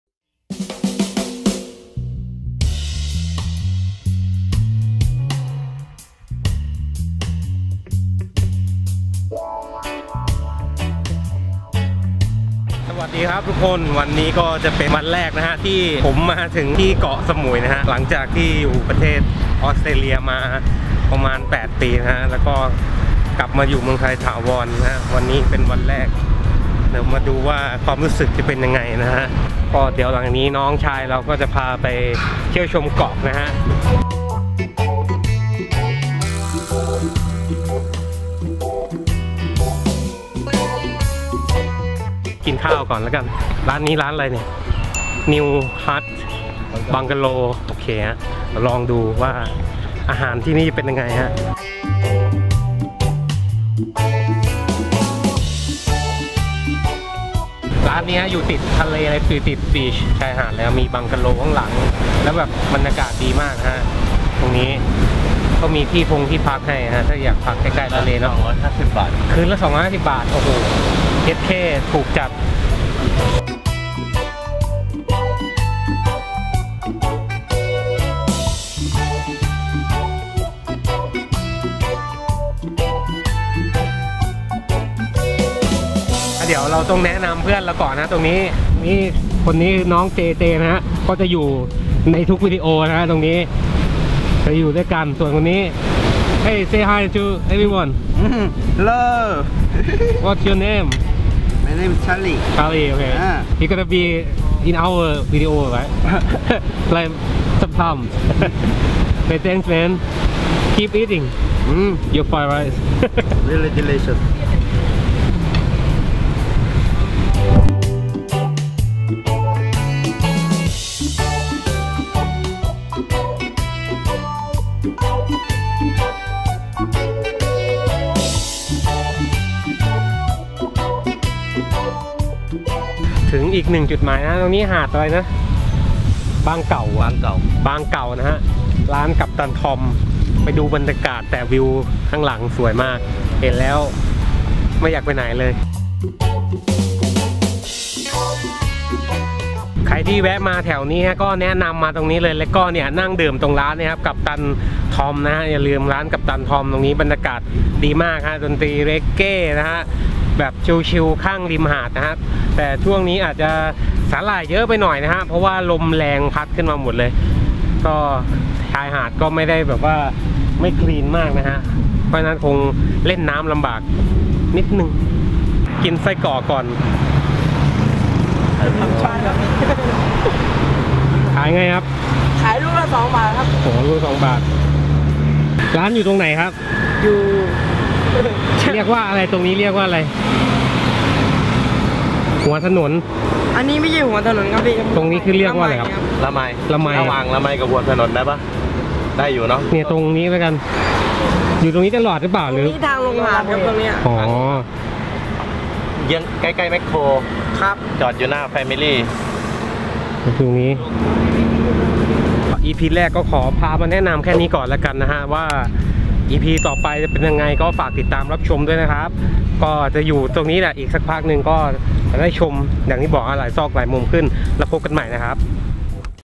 ที่นี่มันสมุยหนิ!! [ ต้าติดเกาะ ep.0 ]
ย้ายกลับมาอยู่ไทยวันแรก หลังจากอยู่ออสเตรเลียมา8ปี มาเป็นชาวเกาะ เลยตัดสินใจทำ EP.ต้าติดเกาะ พวกเราจะพาเที่ยว พากิน พาทำกิจกรรมแปลกแหวกแนวที่น่าสนใจในเกาะสมุยและใกล้เคียง

วันแรกเบาๆก่อนด้วยการพาไปชมรอบๆเกาะ มีอีกหนึ่งช่องทางให้รับชมทางเพจ